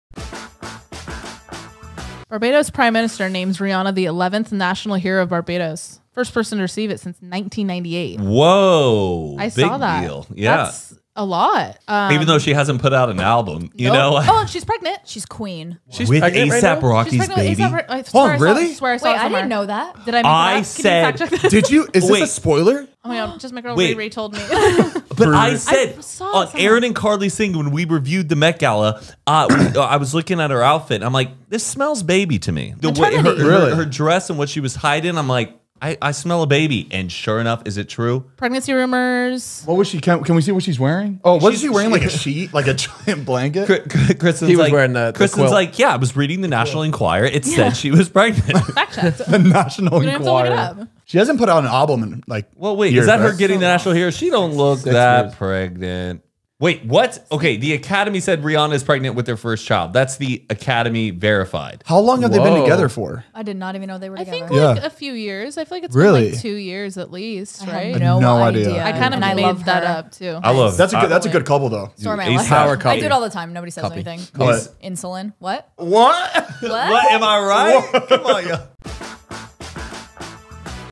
IS RIHANNA SECRETLY PREGNANT?!
With pregnancy rumors swirling, is Rihanna hiding a baby bump??